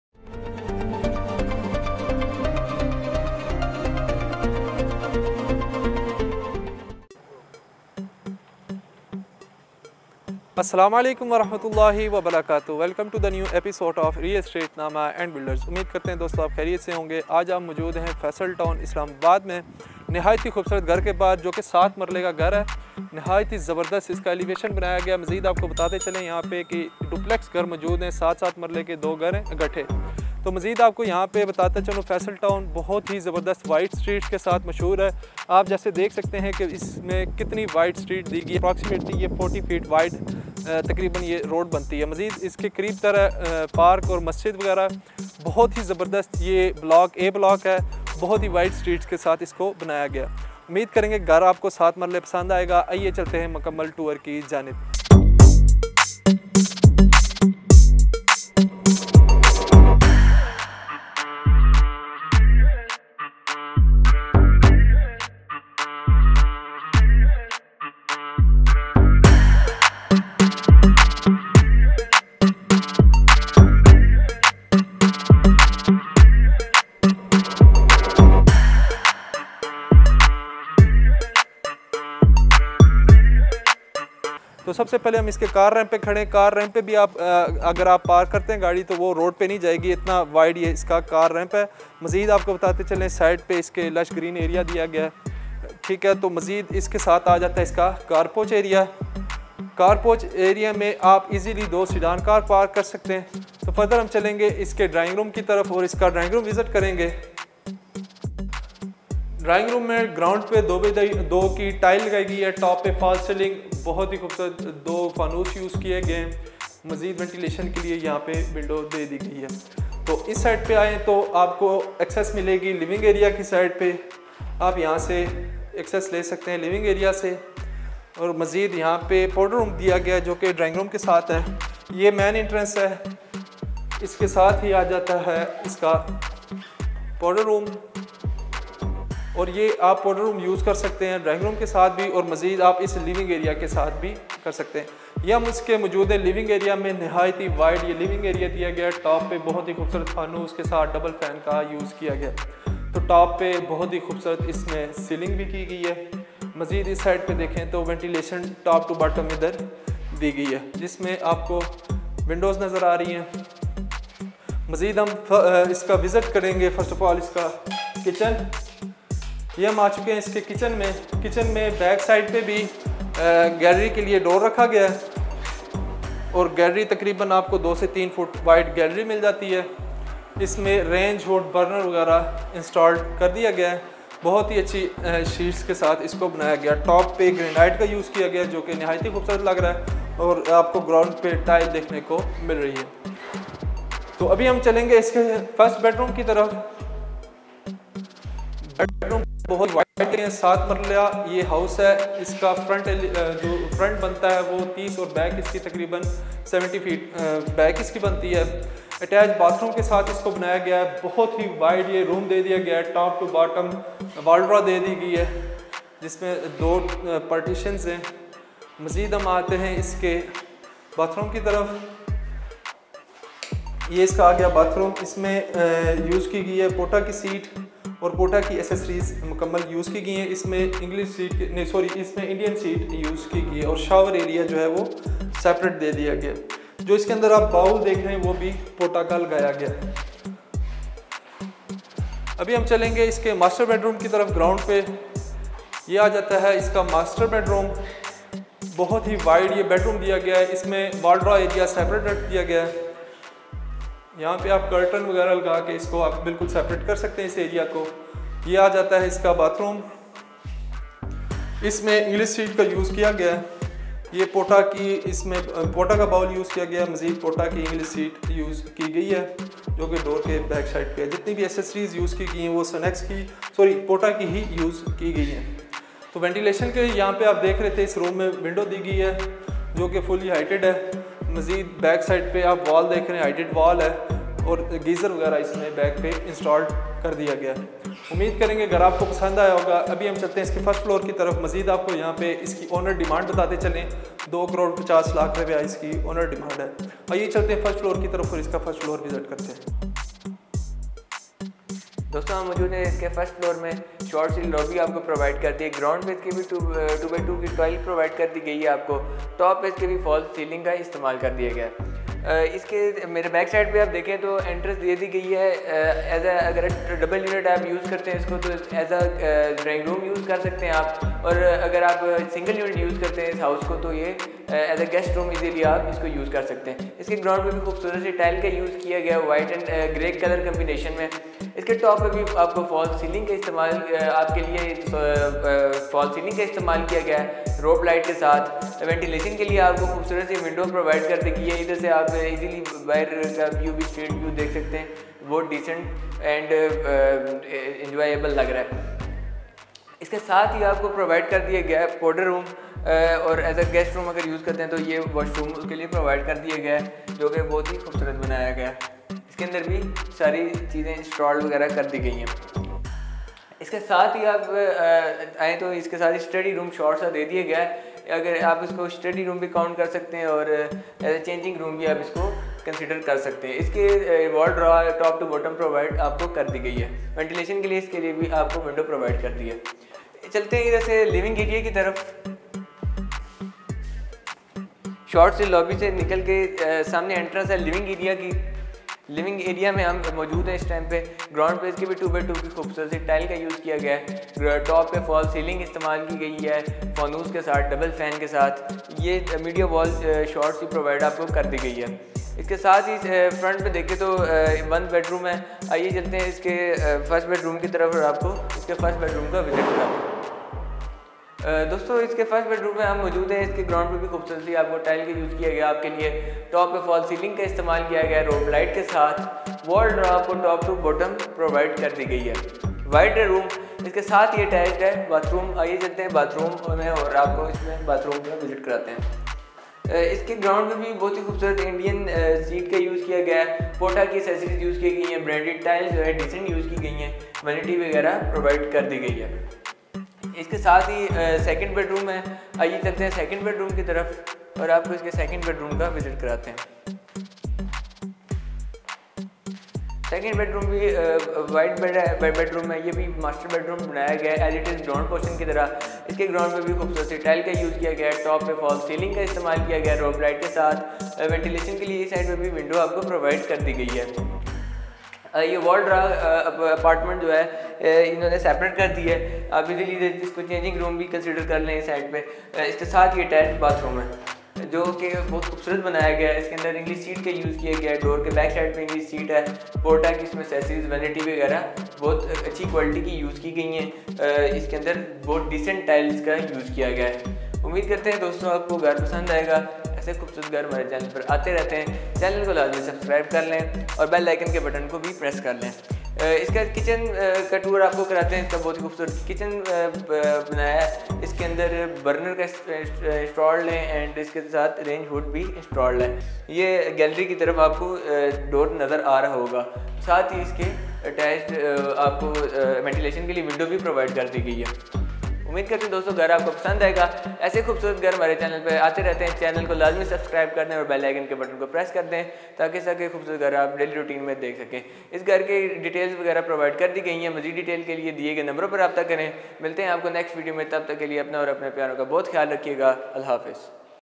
7 Marla House For Sale || Faisal Town || Islamabad || Real Estate Naama || Episode 148 || REN
@Real Estate Naama
7 Marla Brand New  House For Sale In Faisal Town Sector A / Near  By  Islamabad GT Road And Motorway
4 bed double unit \ also Near by Market And Park
@Real Estate Naama
2 bed double unit \ also Near by Market
7 Marla House For Sale || Faisal Town || Islamabad || Real Estate Naama || Episode 148 || REN
We have also other options are available